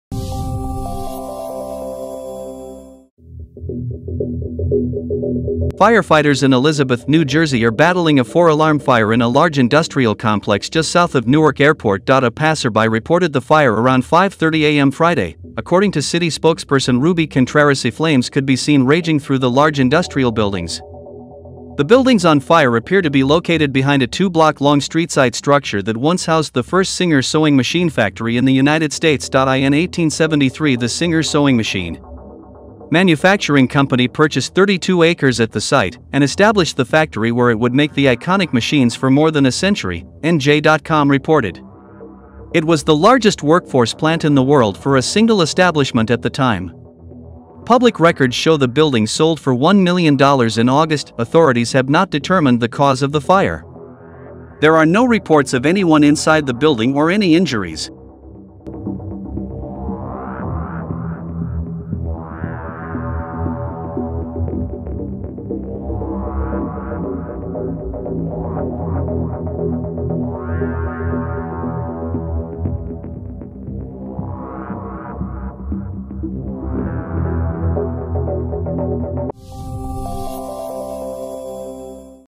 New Jersey firefighters battle 4-alarm blaze at former home to Singer's 1st sewing machine factory
Firefighters in Elizabeth, New Jersey are battling a four-alarm fire in a large industrial complex just south of Newark Airport.

A passerby reported the fire around 5:30 a.m. Friday, according to city spokesperson Ruby Contreras.

Flames could be seen raging through the large industrial buildings.

The buildings on fire appear to be located behind a two block long streetside structure that once housed the first Singer Sewing Machine factory in the United States.

In 1873 the Singer Sewing Machine Manufacturing Company purchased 32 acres at the site, and established the factory where it would make the iconic machines for more than a century, NJ.com reported. It was the largest workforce plant in the world for a single establishment at the time. Public records show the building sold for $1 million in August.

Authorities have not determined the cause of the fire. There are no reports of anyone inside the building or any injuries.